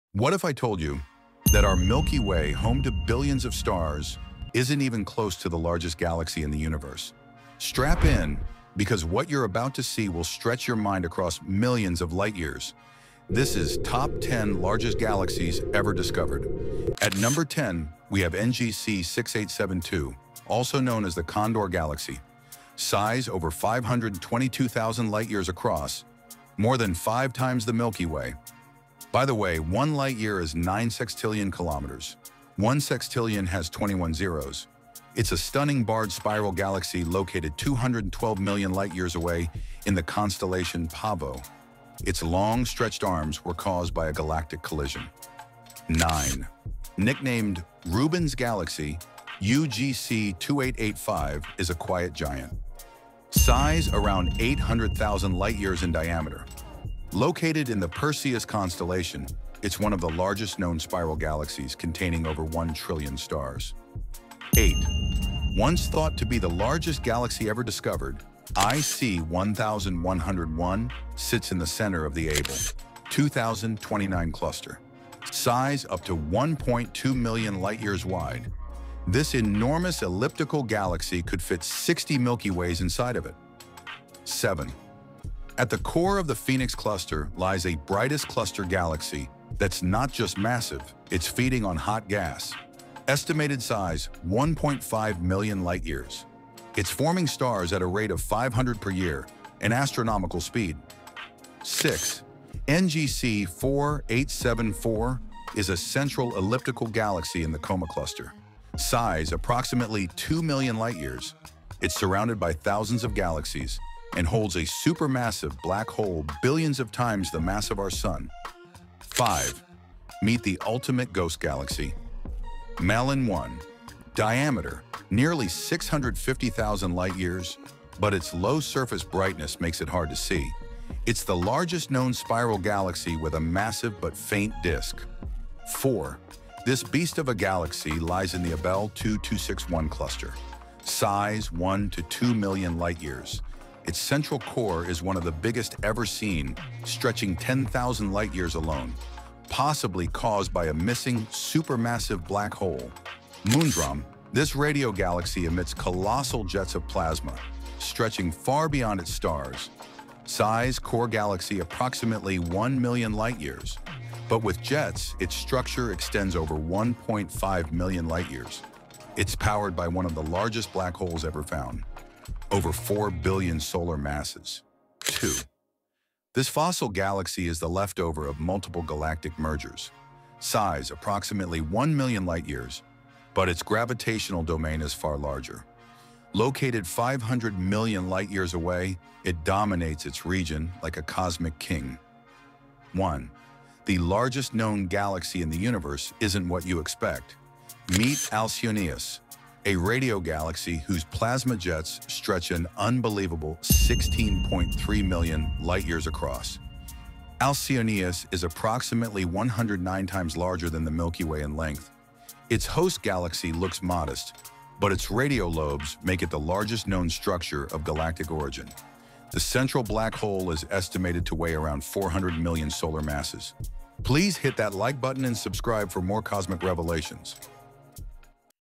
10 largest Galaxies Ever Discovered In The Universe
🌌 Top 10 Largest Galaxies Ever Discovered in the Universe 🌌
Think the Milky Way is massive? Think again. In this jaw-dropping video, we explore the biggest galaxies ever observed — some stretching over millions of light-years across! From spiral giants to elliptical monsters and even radio galaxies with cosmic jet wings, these galaxies redefine what "huge" means in space.

🌀 Discover mind-blowing facts about:
IC 1101 – The former size king
Malin 1 – A ghostly spiral of epic proportions
UGC 2885 – Nicknamed “Rubin’s Galaxy”
Alcyoneus – The current record holder, spanning 16.3 million light-years!

0:00 - Intro
0:16 - 10
0:45 - 9
1:05 - 8
1:24 - 7
1:41 - 6
1:59 - 5
2:16 - 4
2:33 - 3
2:55 - 2
3:13 - 1

🚀 Whether you're a space lover, science geek, or curious explorer — this is your ultimate guide to the largest structures of galactic origin in the known universe.